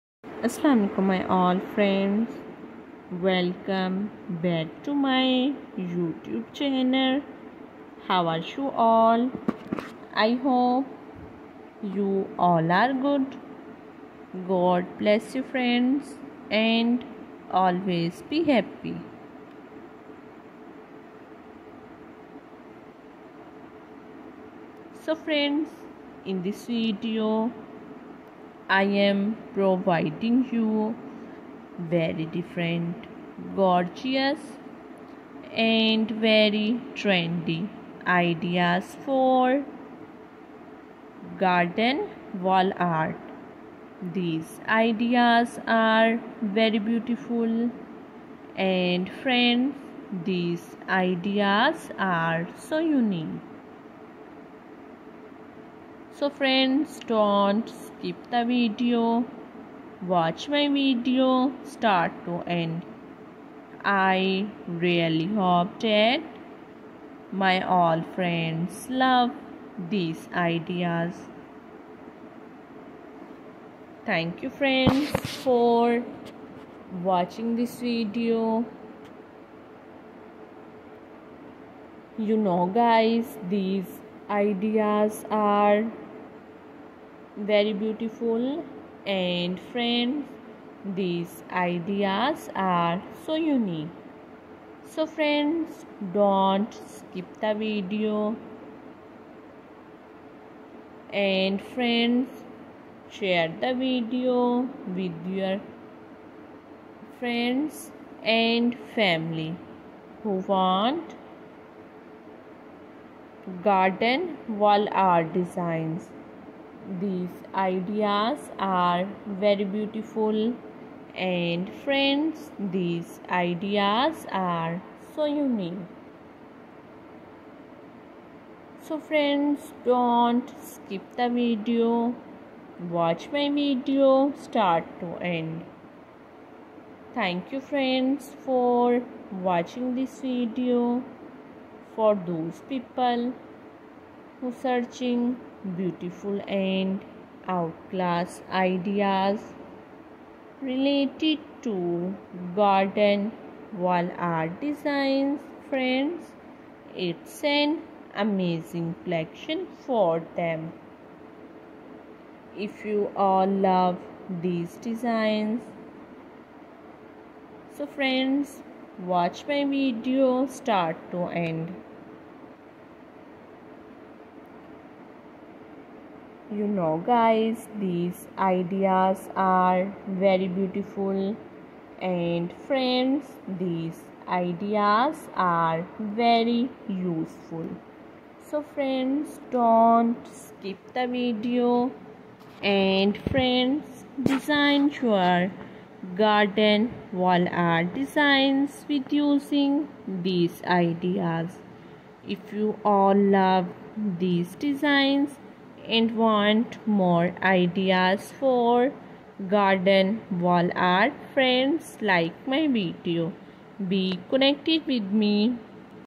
Garden Wall Art Masterpiece - Art in Nature: Beautiful Garden Wall Art Designs
Dive into the world of garden wall art, where we explore stunning, creative ways to transform your garden walls into beautiful, vibrant spaces. From DIY projects to professional installations, we cover everything you need to know to make your outdoor areas come alive with art. Join us for inspiration, tips, and tutorials on adding personality and charm to your garden walls!